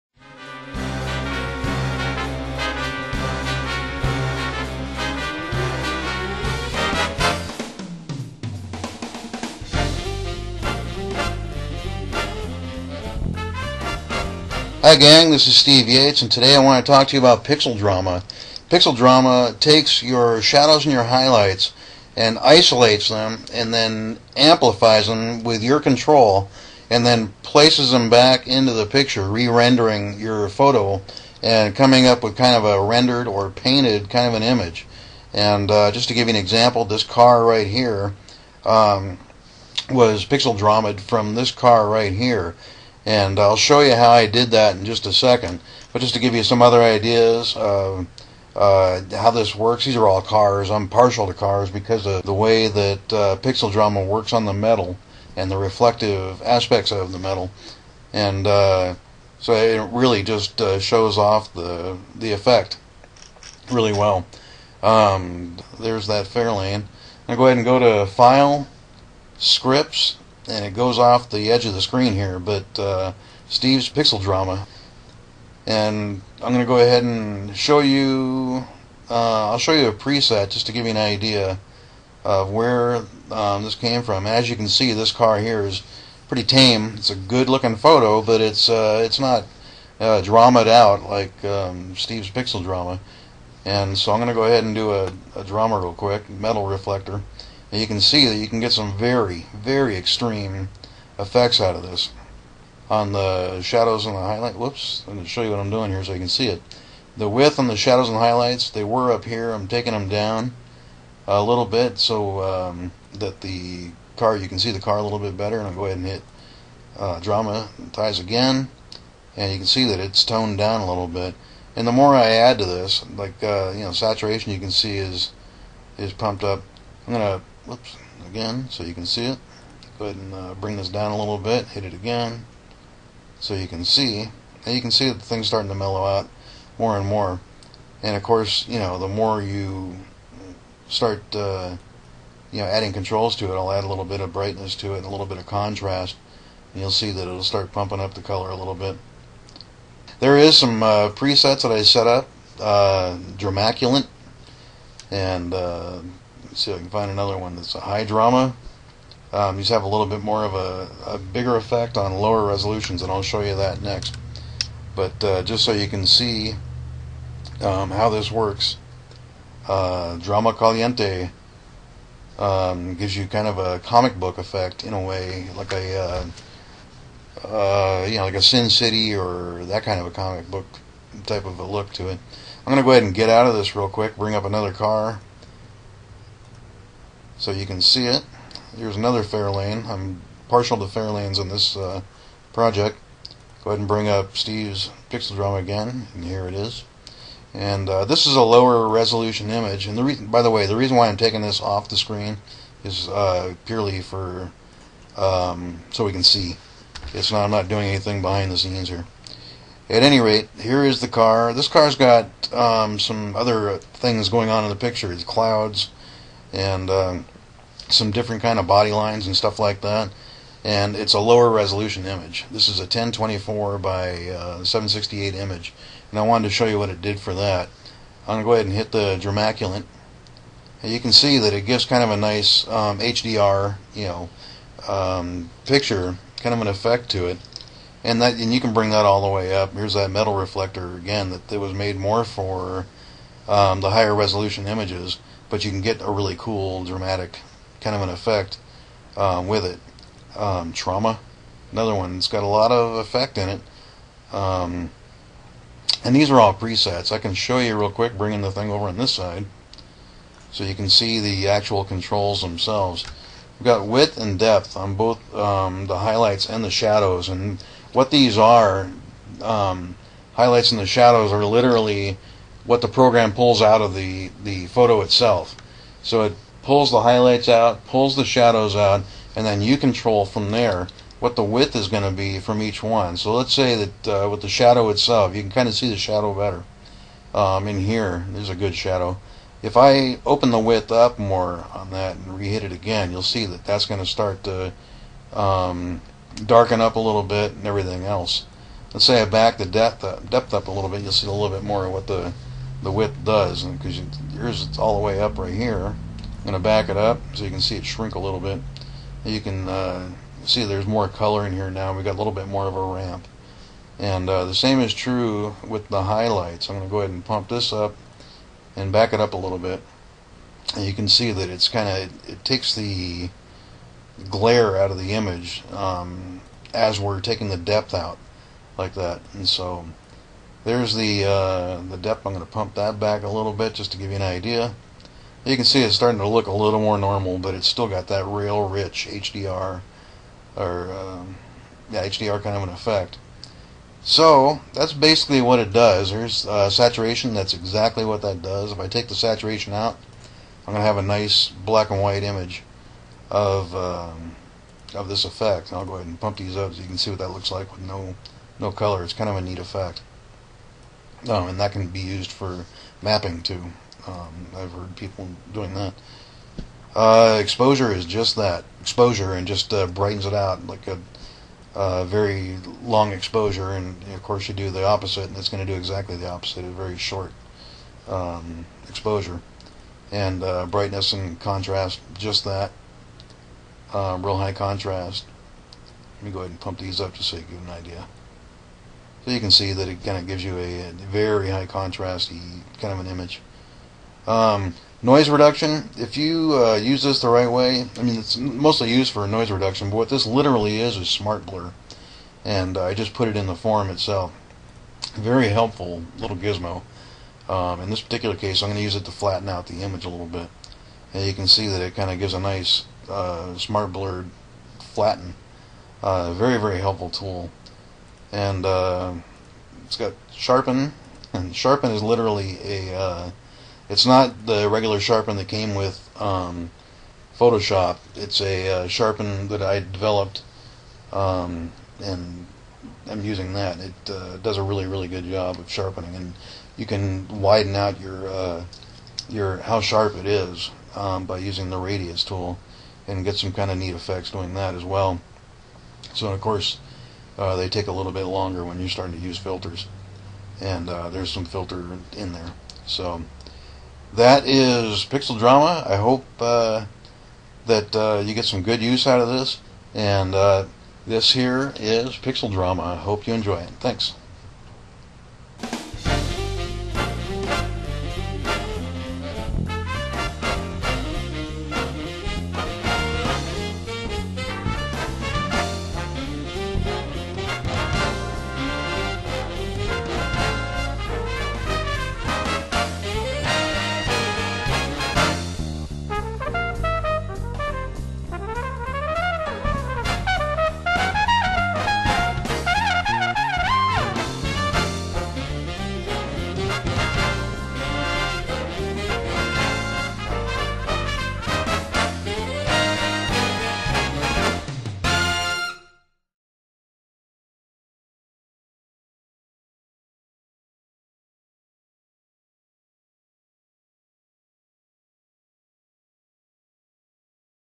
Pixel Drama - Killer Photoshop Effect for CS2 and up!
Re-render your photos and make them look like paintings with Pixel Drama! Pixel Drama isolates your photo's Shadows ans highlights and allows you to control how wide and how deep you want them. The result is anywhere from clear HDR effects to extreem digitally painted renderings. Watch this video to see how!